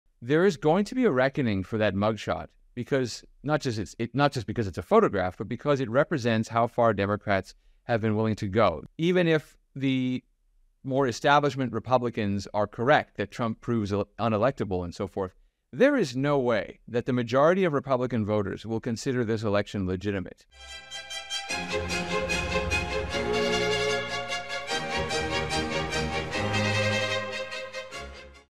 Joel Pollack: No way that Republicans will consider next election legitimate | Caroline Glick Clips
@breitbartnews92 Senior Editor-at-Large Joel Pollak explains the effect that Trump mug shot and trials will have on Republican voters.